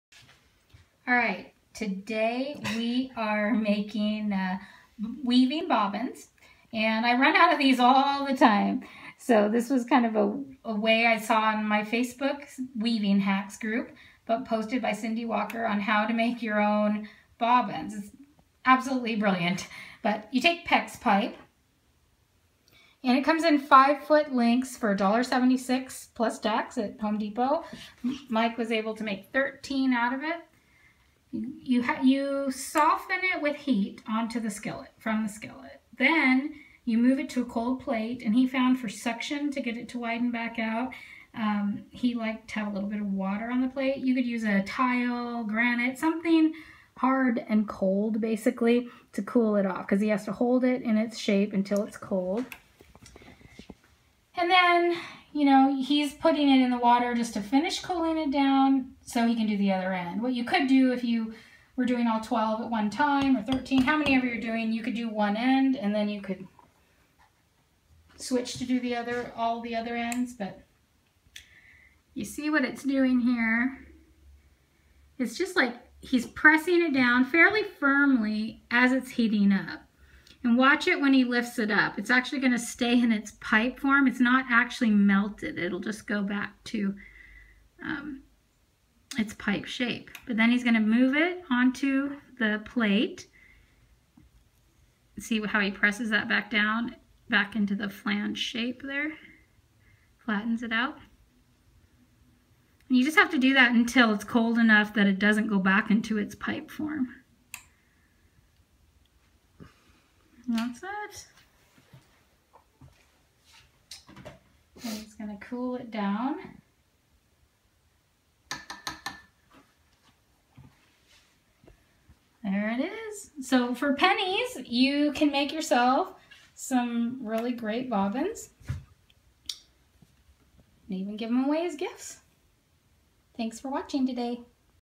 DIY weaving bobbins
Weaving bobbins made from Pex pipe. This idea I got from the Facebook group "weaving hacks". Very easy, fast, & inexpensive to make. Be sure to protect yourself from fumes (ventilator???) Make these in a well ventilated area. Also use an old pan. Something you do not use for food.